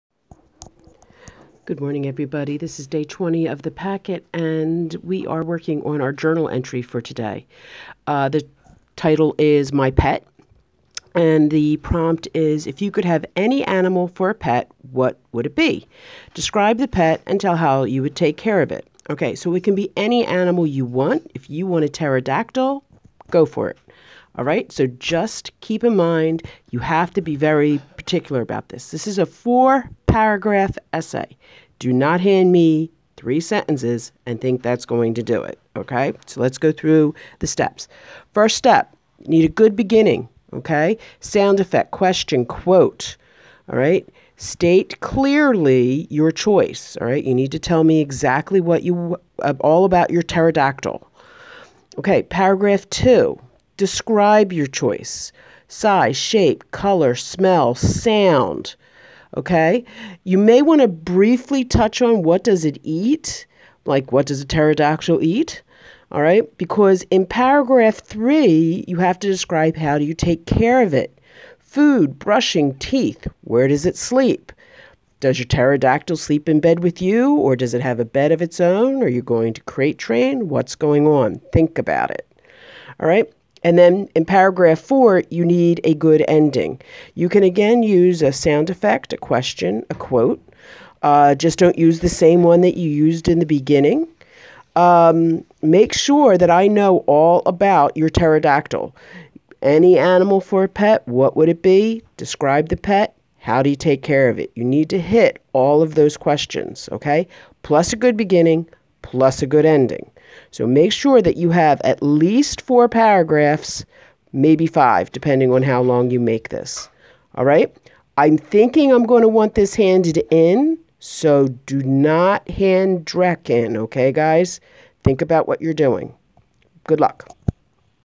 Day 20 of the Remote Learning Packet---My Pet
Ms Grace guides you through the expectations for the journal entry: 4 paragraphs, Good Beginning, Good Ending, Sensory Words, etc. Ms Grace may want this for a grade so be aware of that as you are working.